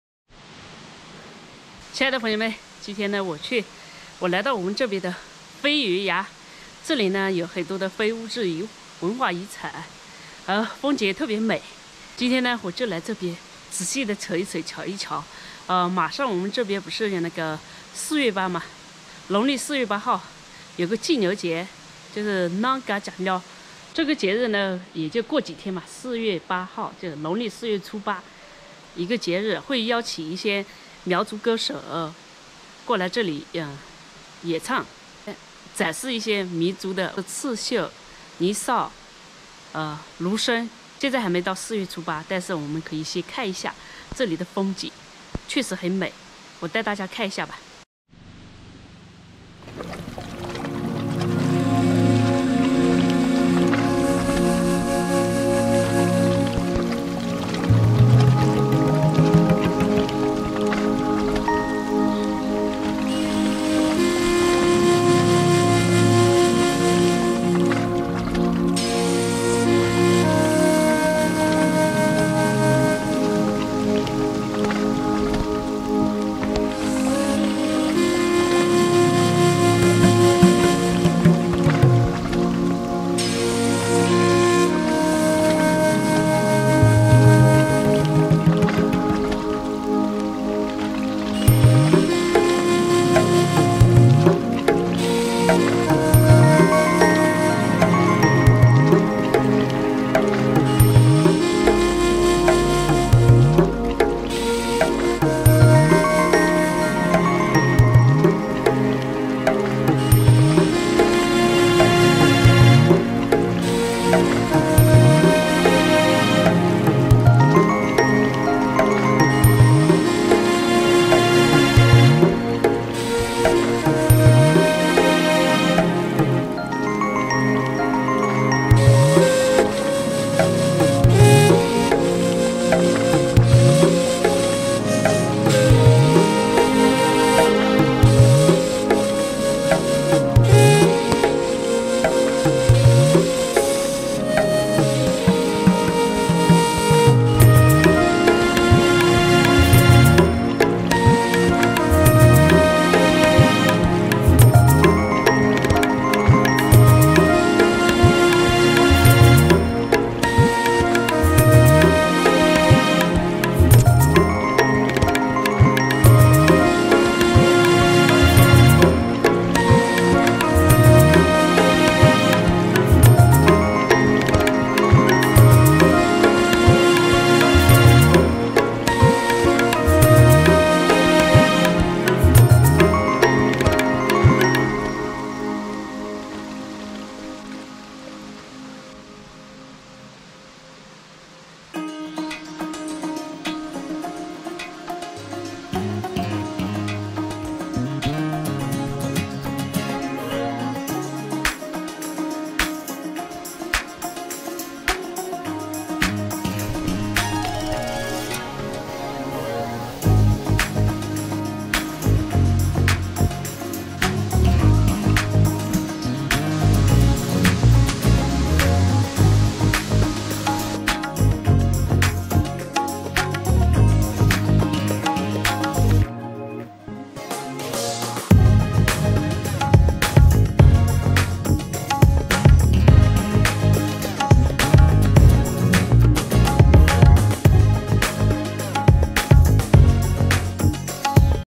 贵州旅游5月值得去的地方，原生态的苗族，神秘的节日值得观赏
大家好，我是苗阿朵💕
吃都吃得没滋没味，怎能活得有滋有味~
阿朵为你分享农村生活，乡村美食做法，吃的就是原生态美食！